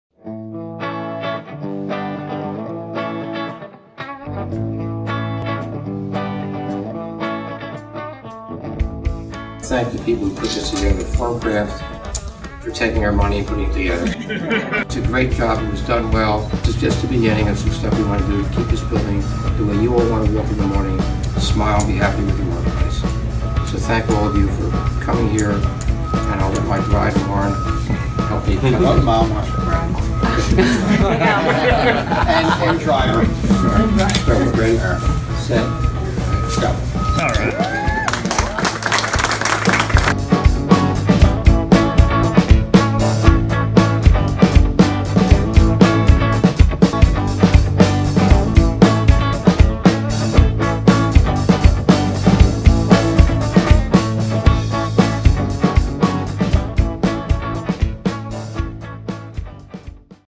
Formcraft - Almo Project
Over the past several years, Almo Corporation has experienced strong growth in the number of employees in its corporate offices located in Philadelphia, PA.

Innovative workplace design strategies were applied to create 7,000 sf of open and inviting space that is also comfortable and productive.